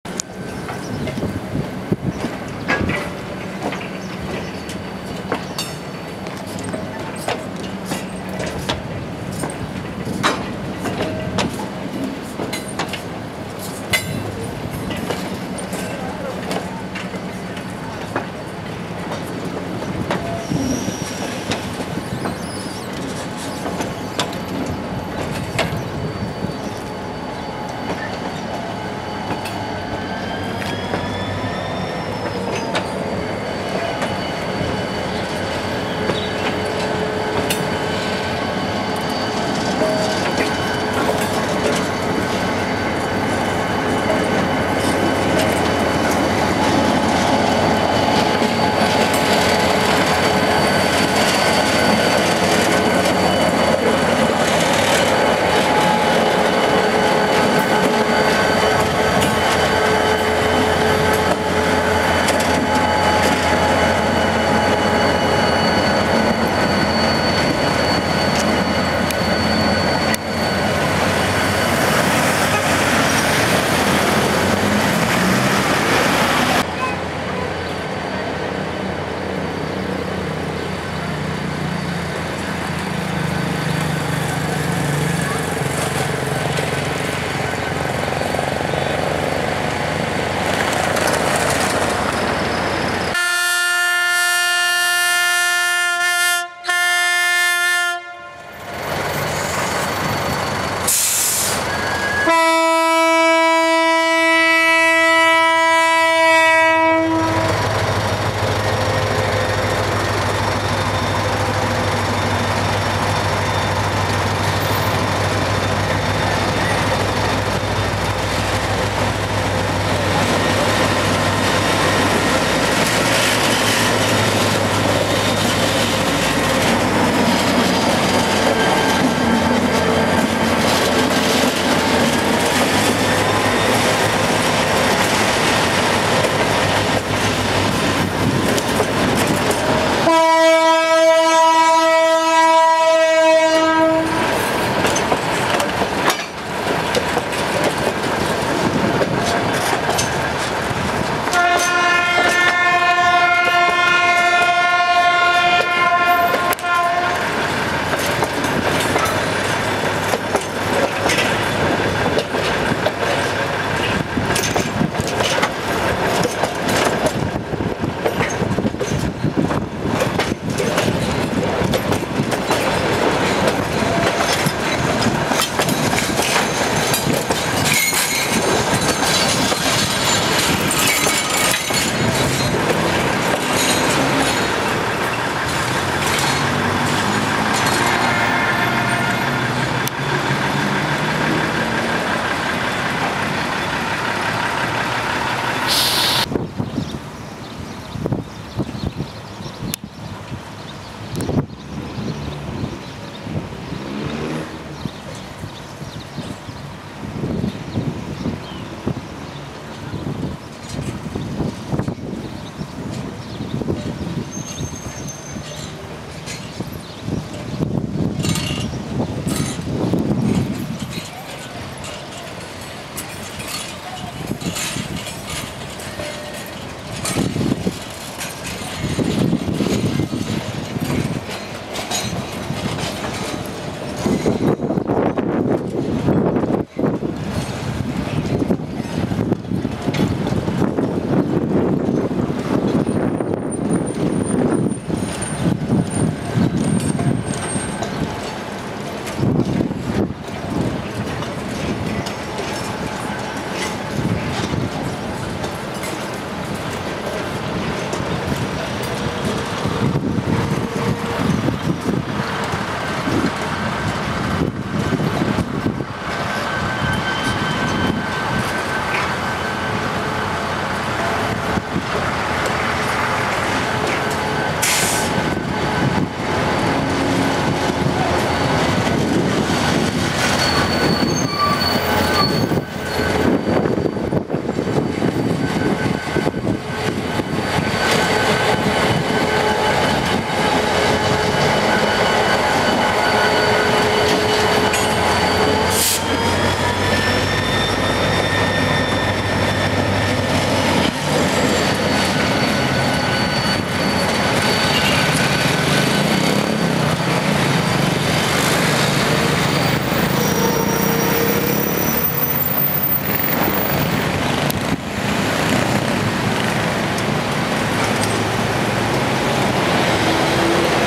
GENERAL MOTORS GT22CU 9731 ARMANDO EL TREN A LAS NUBES
Estación Salta, día miércoles(día previo a la partida del tren a las nubes), pasadas las 17,00 hs, las dos General Motors GT22CU que el Belgrano Cargas asigna a la corrida del tren a las nubes, llegan a Salta provenientes de General Guemes y comienzan con las maniobras de armado de los trenes.
La 9731 lleva a los coches de la formación del tren a las nubes a inspección, luego saliendo al cambio externo a la estación, cambia de vía al tren a las nubes para empujarlo hacia el anden N°1 de estación Salta.
Formación del tren a las nubes:
Locomotora: General Motors GT22CU # 9731
Vagón Sanitario: P.V. 2404
Coches Primera: P.4075-P.4063
Coche Restaurant: R.A.1932
Primera Bar: P.B.4506
Coches Primera: P.4079-P.4072-P.4022
Todos los coches llevan boguies Minden-Deutz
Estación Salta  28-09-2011